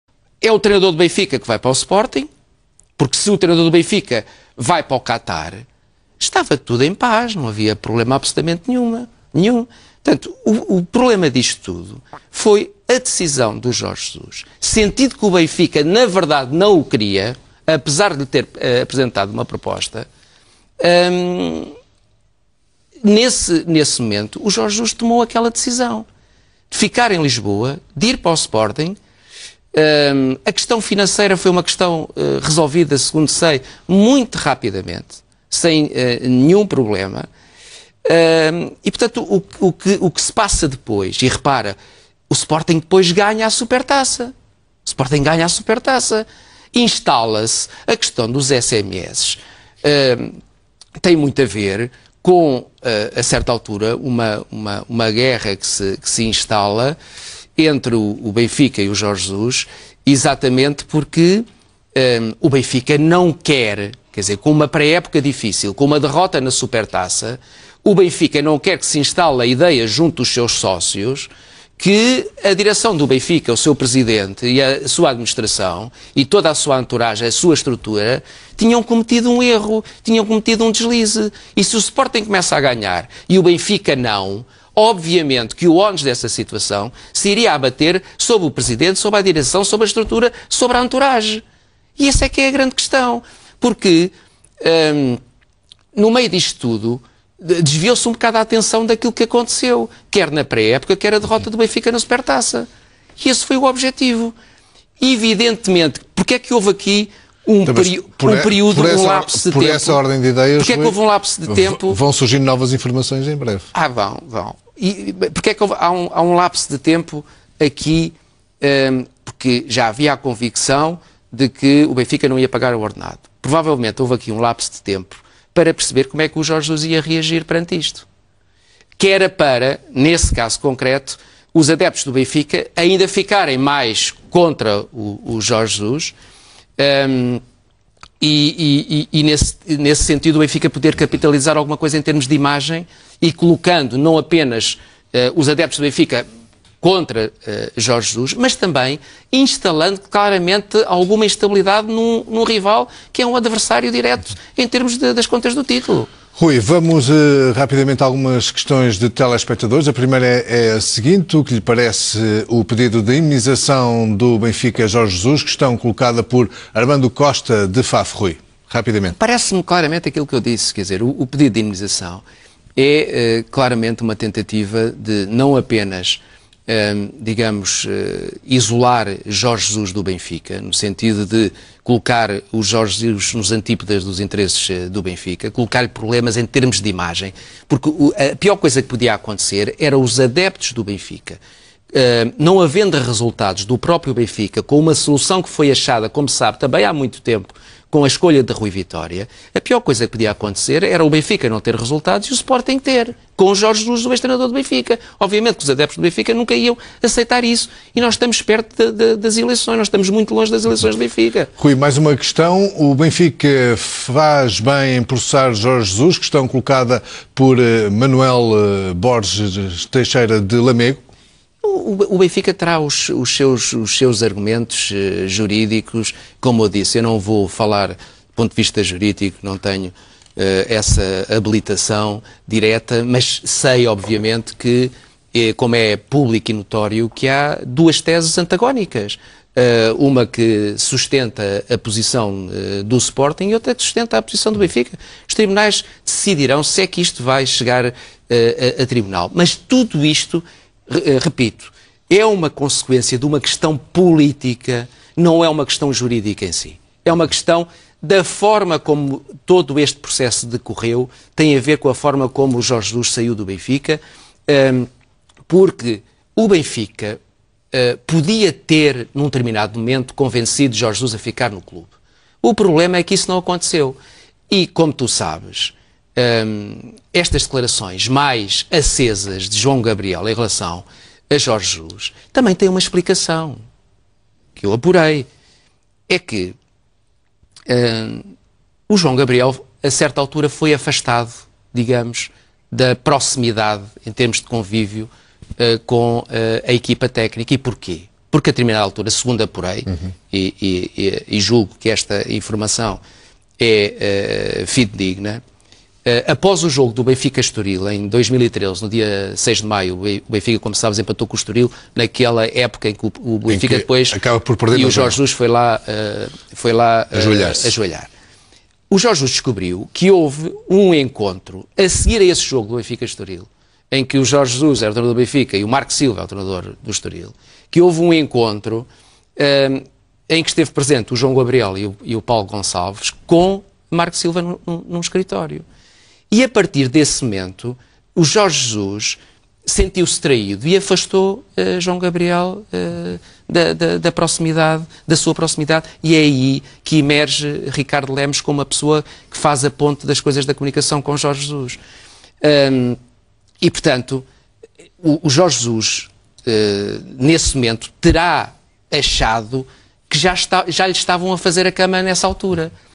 As verdadeiras razões por detrás da polémica dos SMS e do processo a Jorge Jesus (18/8/2015)
Explicação das verdadeiras razões e estratégia por detrás da polémica dos SMS criada pelo Benfica e do processo a Jorge Jesus